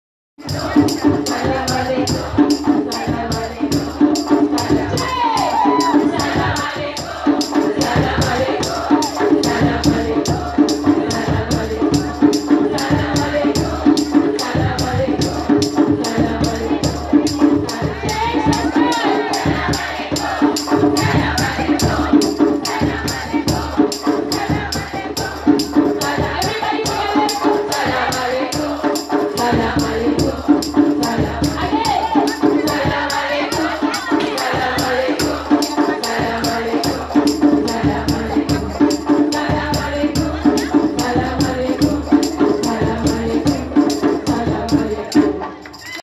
Wanny Music & Dance Therapy More than 1000 women Sama Music Festival Karthoum 2016
Wanny Angerer in Moving Cultures